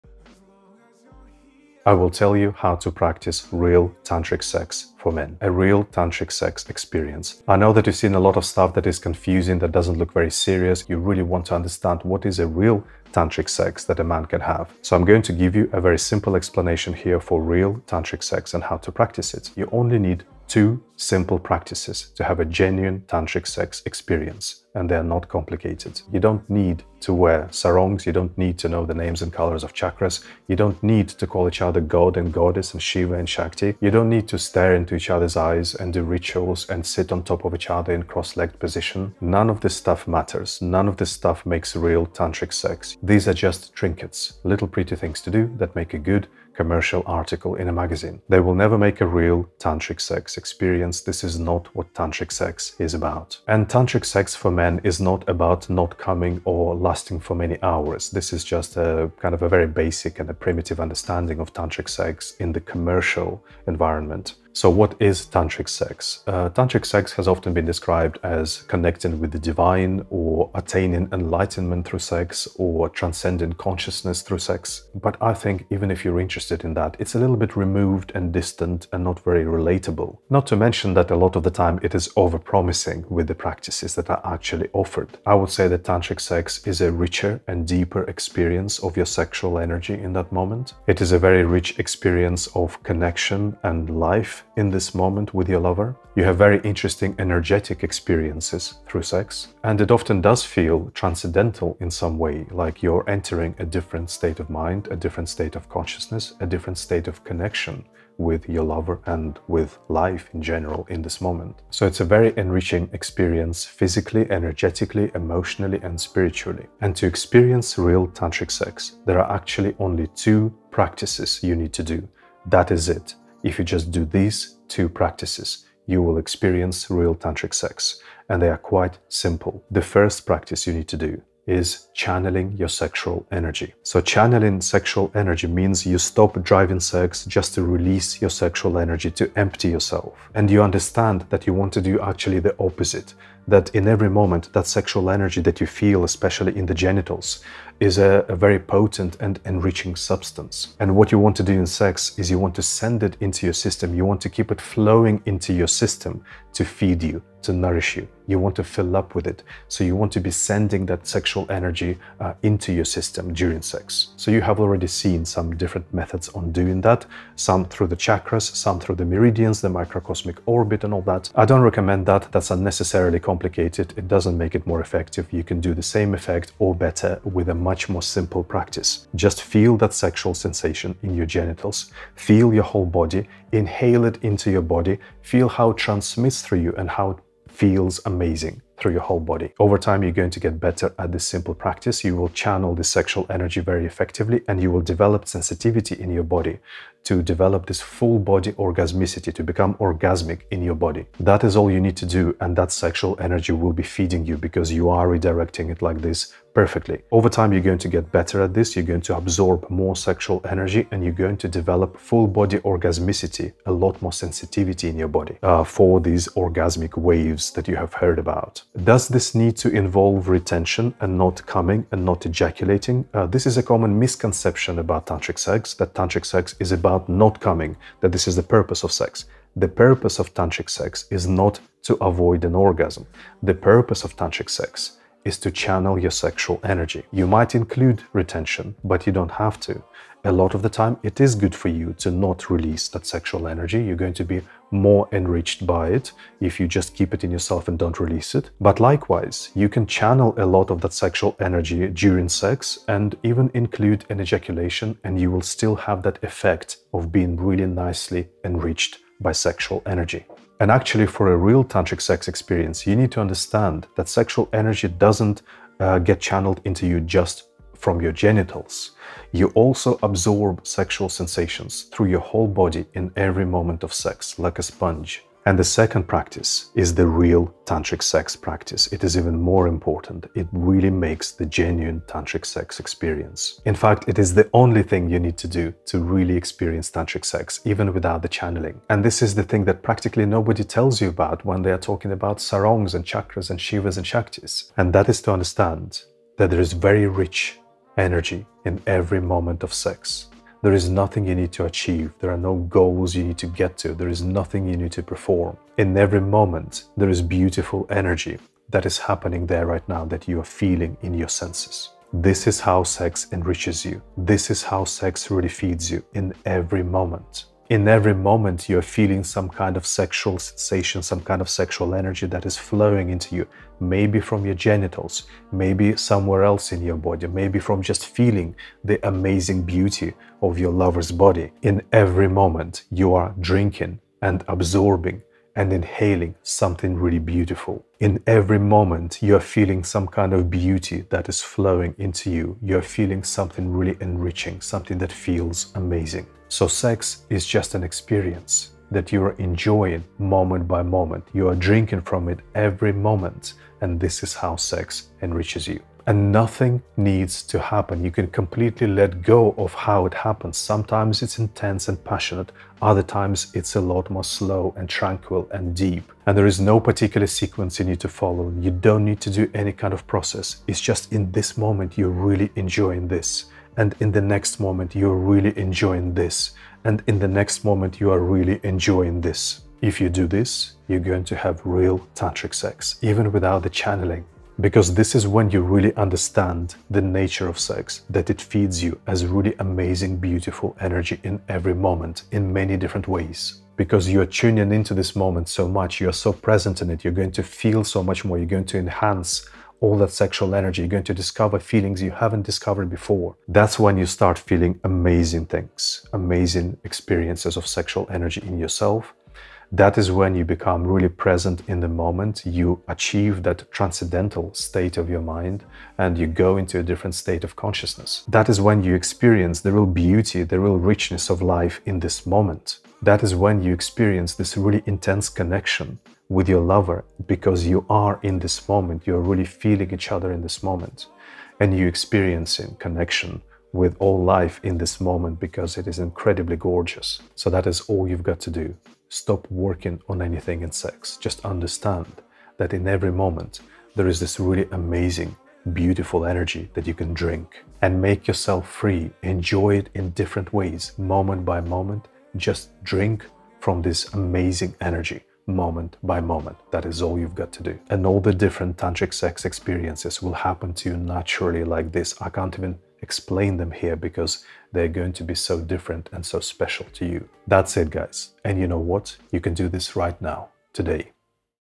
2 simple Tantric sex practices for men for genuine Tantric experience | Alexey Welsh
What is real Tantric sex for men? You only need 2 simple practices. FREE GUIDE TO IMPROVE SEX

In this video I will tell you how to practice real Tantric sex, for a genuine Tantric experience. You actually need to do only two simple practices

I will deliver to you useful advice on sexuality every week.

I am a coach of holistic sex for the last 20 years, specialising in female sexuality.